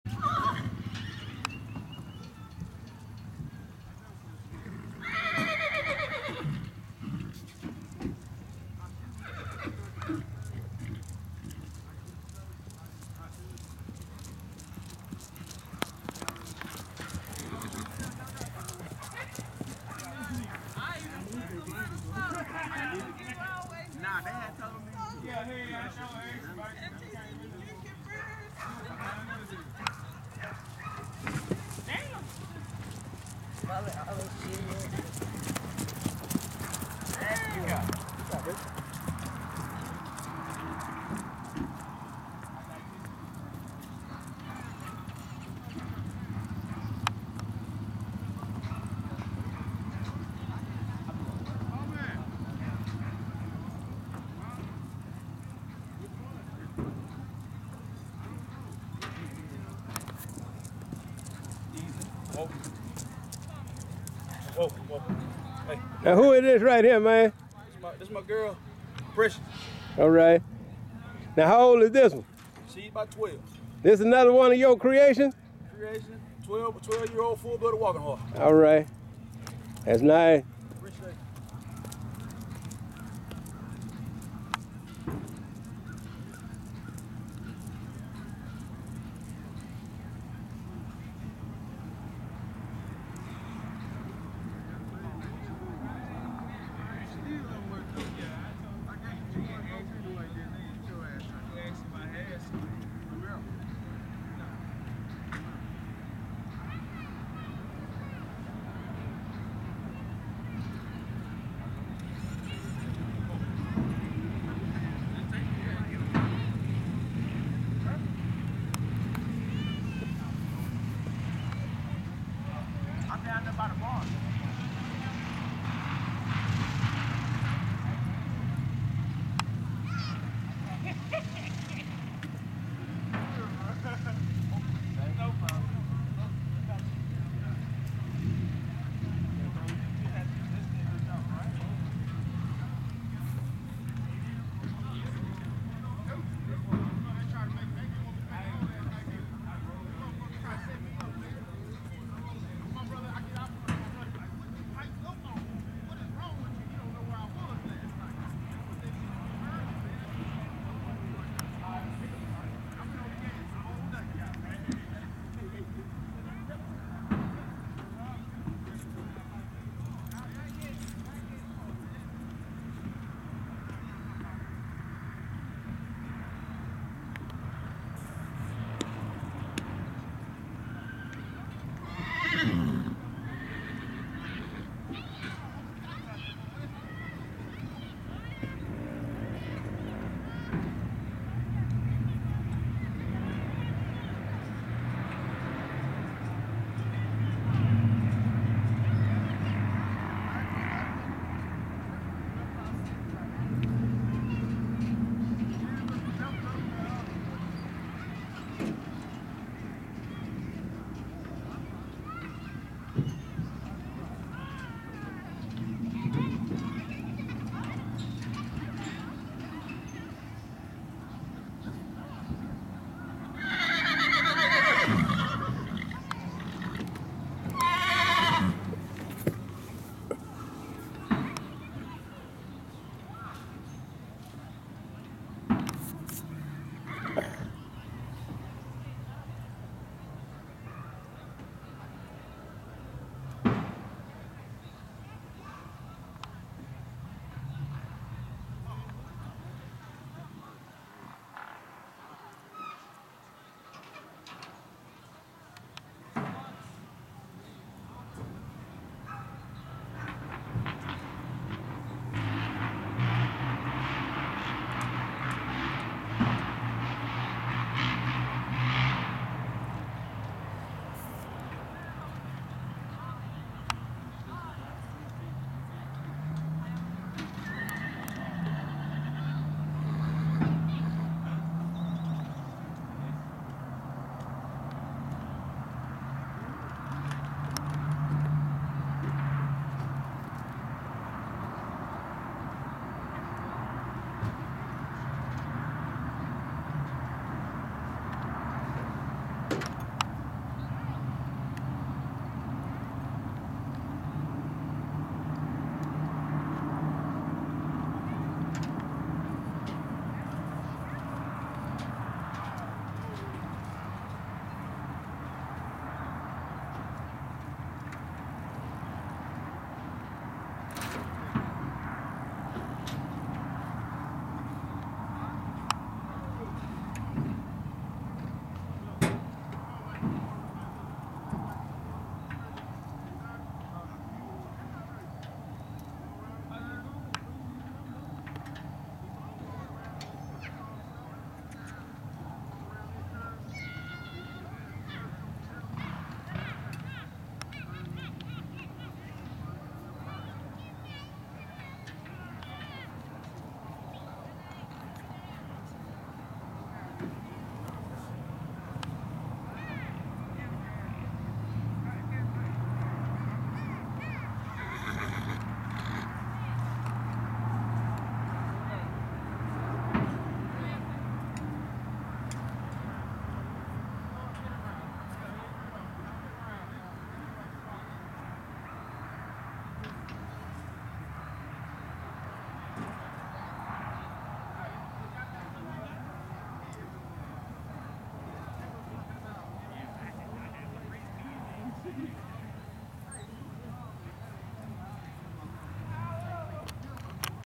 Black Mare Tennessee Walking Horse of Hope Arkansas
Mr. Trotter on his 12 year old mare in Oklahoma